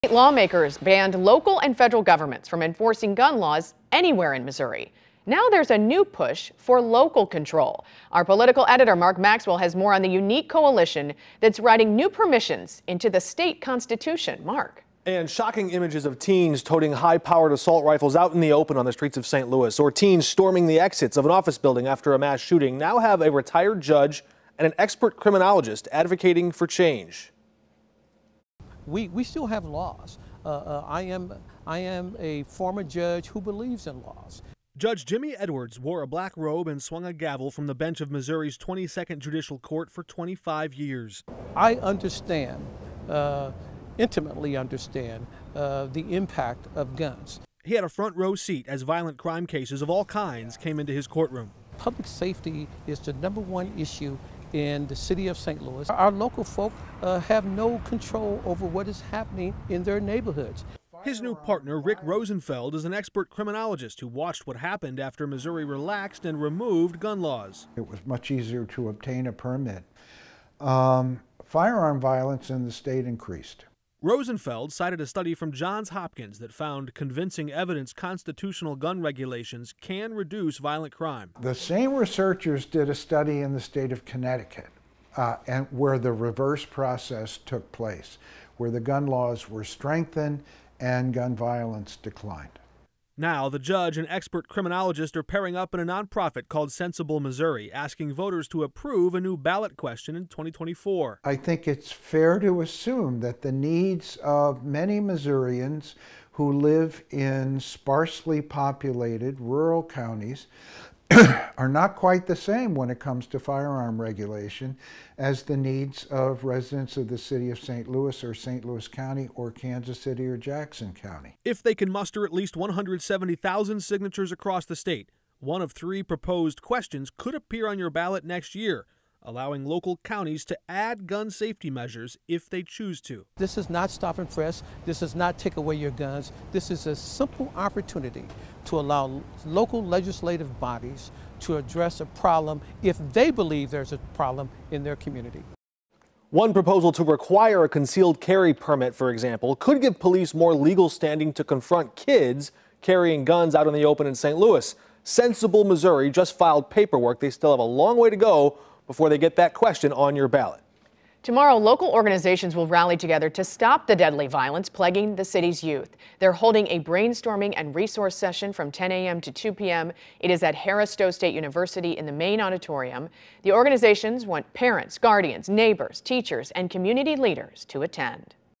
Missouri ballot proposals aim to put gun control in hands of local government
One proposal to require a concealed carry permit could give police more legal standing to confront kids carrying guns out in the open in St. Louis. Sensible Missouri just filed paperwork.